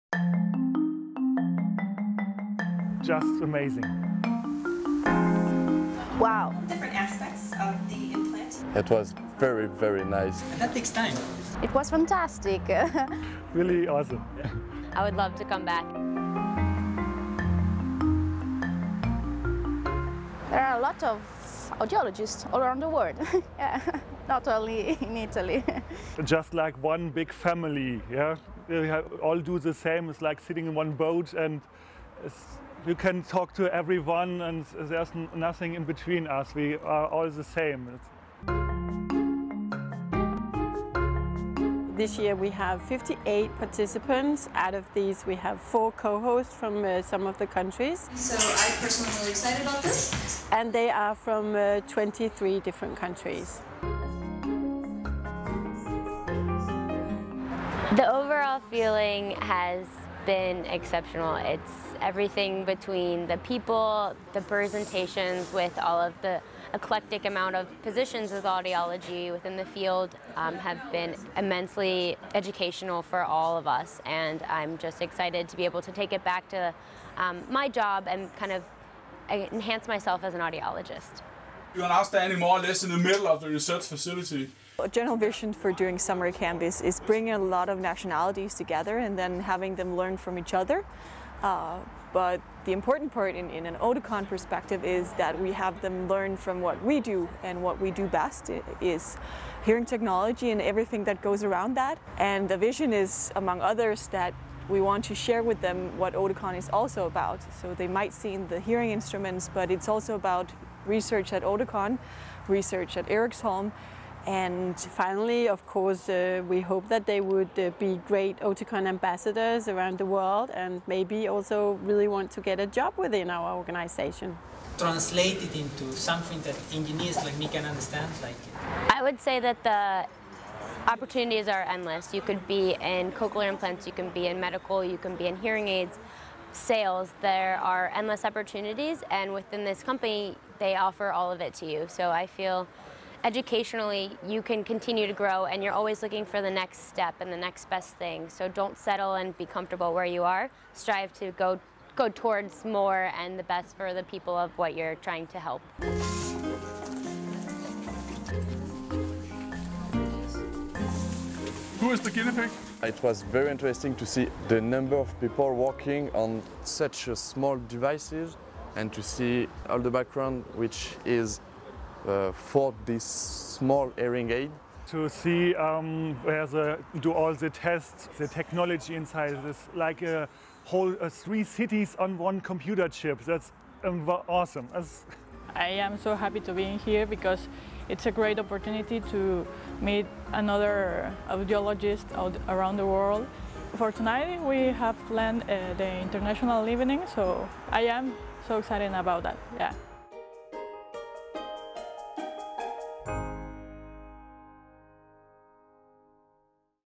Oticon Summer Camp 2016
The Eriksholm Summer Camp

Eriksholm Research Centre hosts the Eriksholm Summer Camp. The main purpose of the Eriksholm Summer Camp is to create a learning environment for young audiologists to work together in a partnership of enthusiasm and experience within the framework of Oticon’s philosophy. Our goal is to provide a memorable experience and a network of friends and colleagues around the world. The programme is therefore a combination of lectures and social events.